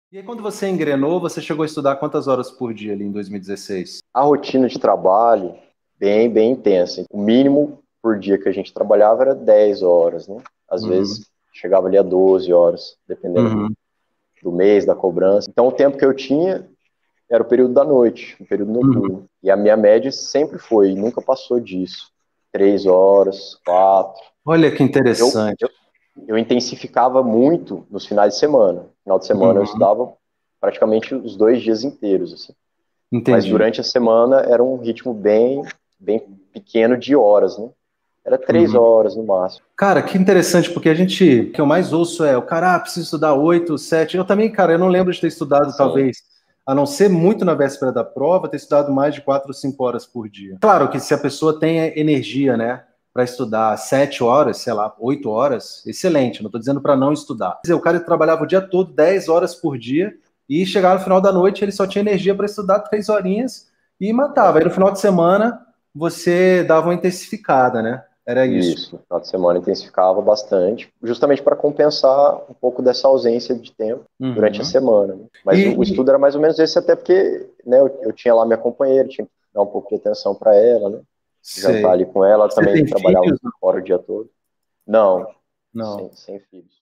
DELEGADO explica como conseguia conciliar trabalho e estudo [Cortes VSD]
Achou pouco tempo de estudo por dia? Diga nos comentários 🙂